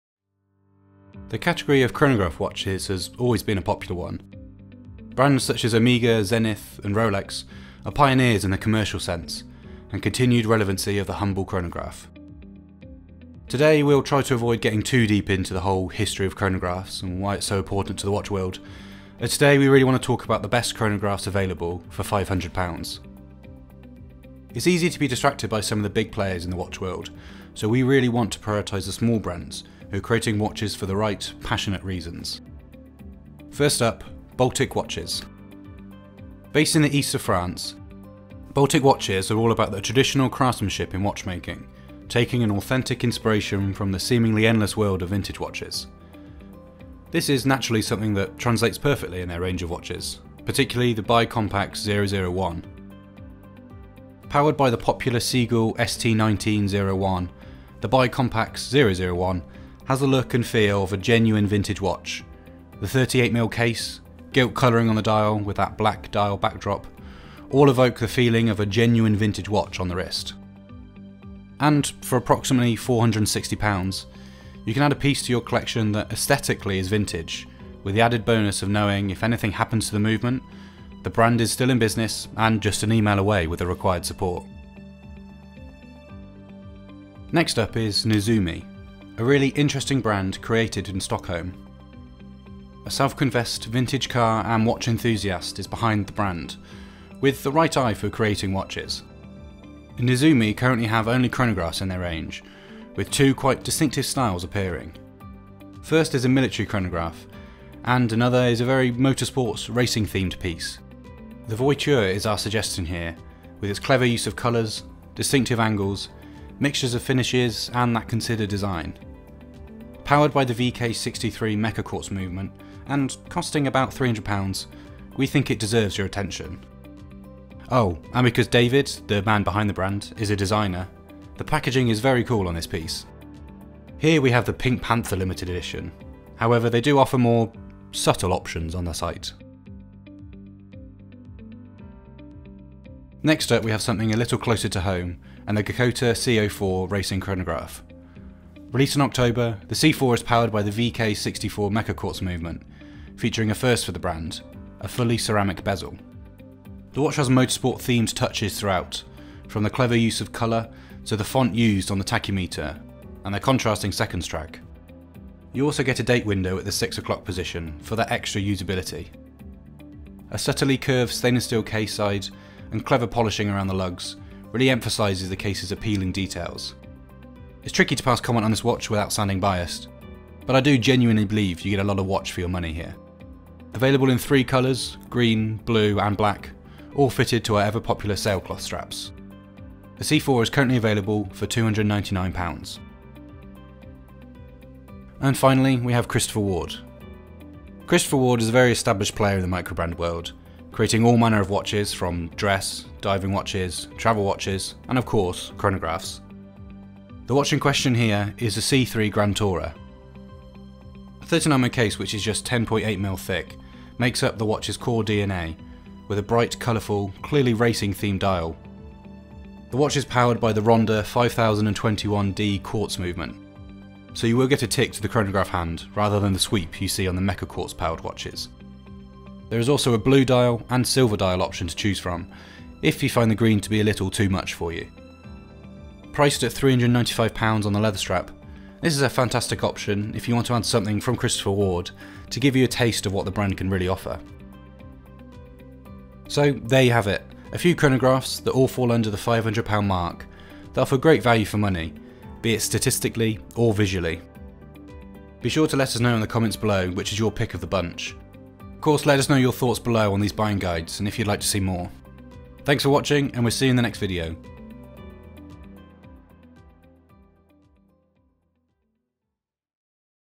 The Best Chronograph Watches For £500! | A WatchGecko Buying Guide
WatchGecko

*Model Wrist Sizes*
7 inches

Products featured;

Geckota C-04 VK64 Space Age Racing Watch Gift Set

Discover some of the best chronographs available online up to £500 from interesting new brands...
The category of chronograph watches has always been a popular one. Brands such as Omega, Zenith, Rolex and Tag Heuer are pioneers in the commercial success and continued relevance of the humble chronograph. We will try to avoid getting too deep into the whole history of chronographs and why they’re so important to the watch world (especially chronographs with an automatic movement...another time maybe) as today we want to talk about the best chronographs available today for £500 (and under).

Today we’re focusing on watches that punch well above their price tag from the world of watches. It’s easy to be distracted by some of the big players in the watch world so here we want to prioritise the small brands who are creating watches for the right passionate reasons.

The Baltic BiCompax 001
Based near Besançon in France, Baltic watches are all about the traditional craftsmanship in watchmaking taking an authentic inspiration from the seemingly endless world of vintage watches. This is naturally something that translates perfectly in their range of watches, particularly the BiCompax 001. Powered by the popular Seagull ST1901, the BiCompax 001 has the look and feel of a genuine vintage watch. The 38mm case, gilt colouring on the dial with that black dial backdrop all evoke the feeling of a genuine vintage watch on the wrist.

And for approximately £460 (converted from euros) you can add a piece to your collection that aesthetically is vintage with the added bonus of knowing if anything happens to it (as any vintage chronograph owner knows) the brand is just an email away with the required support.

The Nezumi Voiture

Nezumi Studios is a really interesting brand created in Stockholm by David Cardenes. Self-confessed vintage car and watch enthusiast the designer by trade clearly has the right interests (and eye) for creating watches. Nezumi currently has only chronographs in their range with two quite distinctive styles appearing. First is a military chronograph feel in the form of their Corbeau and the other is a very motorsports, racing-themed 60s era feel in the shape of the Voiture and Loews.

The Voiture is our suggestion here with its clever use of colours, distinctive angles, mixture of finishes and considered design. Powered by the VK63 mecha quartz movement and costing approximately £300 we think it deserves your attention. Oh, and because David is a designer the packaging is very cool on this piece, it’s the small things right?

Geckota C-04 Racing Chronograph

Next up we have something a little closer to home, and the Geckota C-04 Racing Chronograph. Released last month, the C-04 is powered by the VK64 mecha quartz movement featuring a first for the brand; a fully ceramic bezel. The watch has motorsports themed touches throughout from the clever use of colour to the font used for the tachymeter and the contrasting seconds’ track. You also get a date window at the 6 o’clock position for extra usability. A subtly curved stainless steel case side and clever polishing around the lugs emphases the case’s appealing details.

It’s tricky to pass comment on this watch without sound bias, but I genuinely believe you get a lot of watch for your money here. Available in three colours green, blue and black all fitted to our ever-popular sailcloth straps. The watch has 20mm lugs as well-meaning we have plenty of options, just look at 20mm watch bands here!